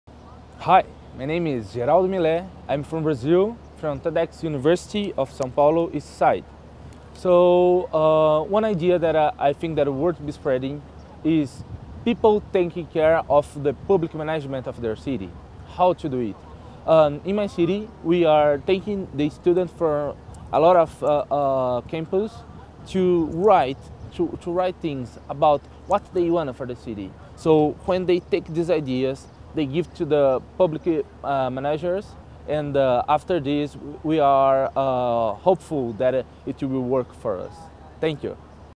59 Geraldo Milet - people taking care of the public management of their city
59 Geraldo Milet - Sao Paulo Brazil - people taking care of the public management of their city
Sonja and I asked some Fellow TEDxOrganizers to share their Idea worth spreading. If you like one of this ideas, please share it.

film equipment for support